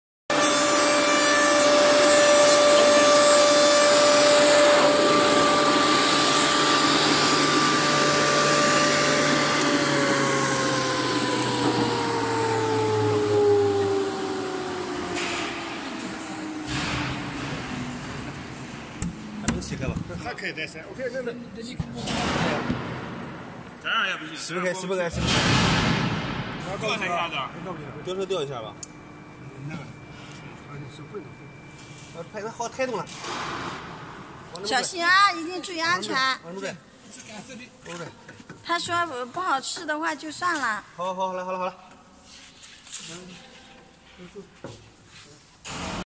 WPC door edge milling machine
This WPC door edge milling machine is used for milling at the edge of door panel to make deeper groove for putting edge covering profile, it can be used for the four sides of door panel